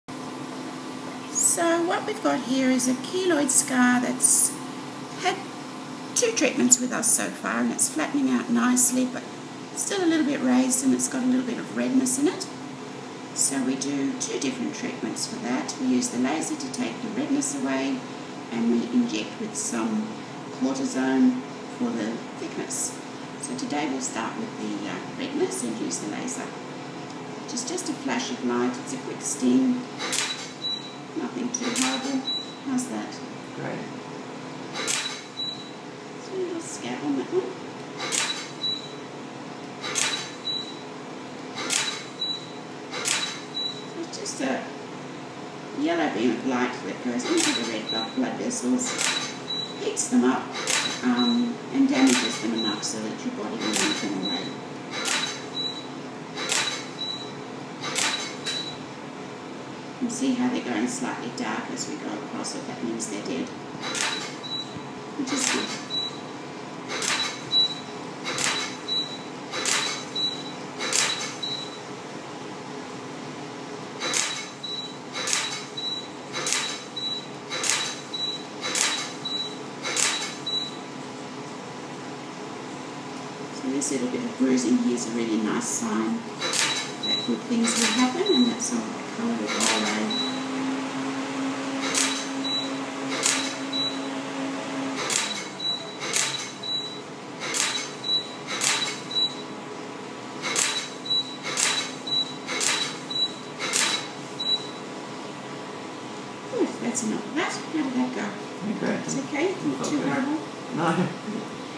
Laser for keloid scars
Persistent redness in keloid scarring is treated using the  Candela V-beam™ laser, which is the same laser  used to  remove blood vessel blemishes such as capillaries, veins  and birthmarks. The Most scars will be red during the healing phase and in most cases this will fade spontaneously. Persistent redness in scars may respond to Candela V-beam™ laser treatment, the same laser used to remove blood vessel blemishes and birthmarks. Beam is the gold standard for improving  redness in keloid scars and  is often used in conjunction with cortisone injections that them flatten.